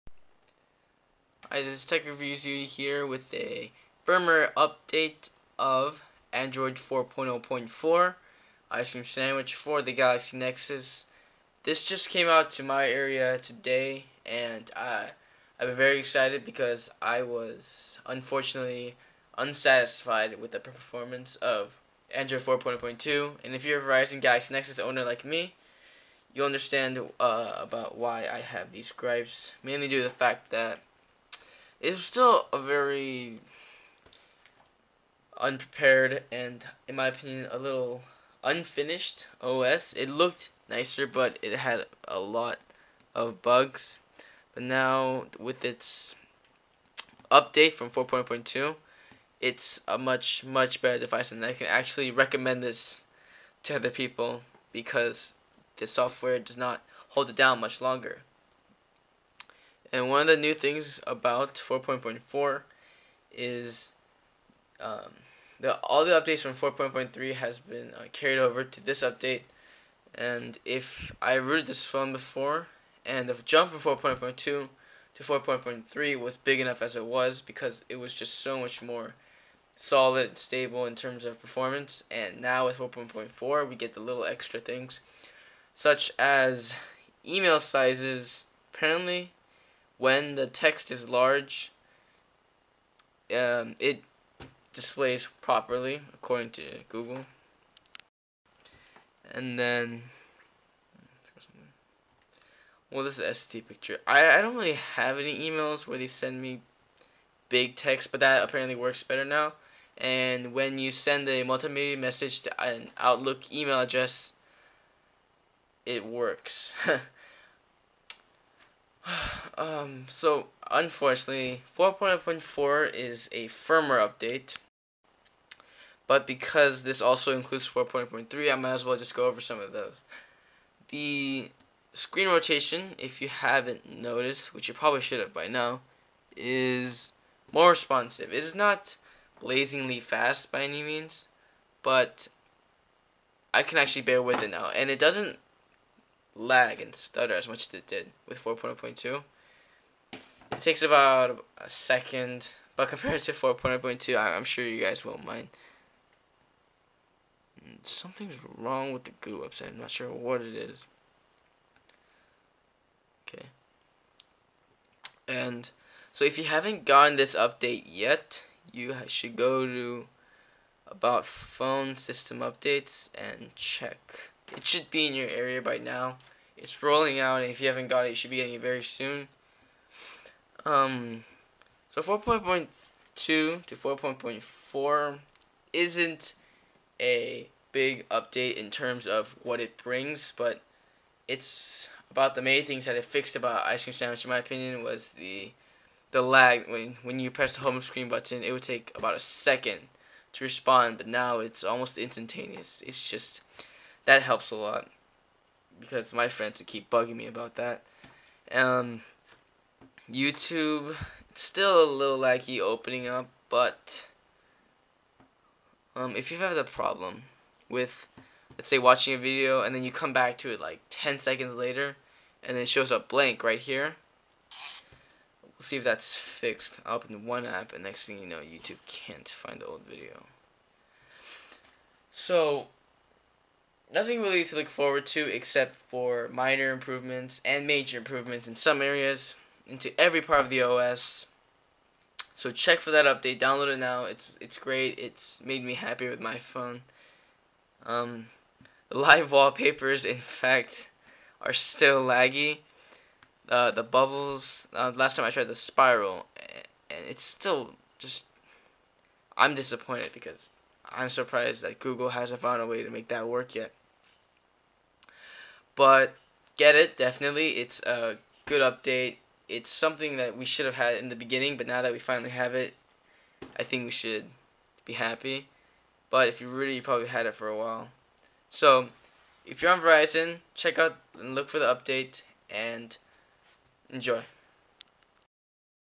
Android 4.0.4 (Official) for Verizon Galaxy Nexus
A new release of Ice Cream Sandwich, 4.0.4, was released on June, 1, 2012, as build IMM76K. Verizon Galaxy Nexus devices jumped from 4.0.2 to 4.0.4 so the more substantial update of 4.0.3 is given to us as well. The two updates bring more stability, bug fixes, and other app updates. This is probably going to be in your area soon so you should check it out now.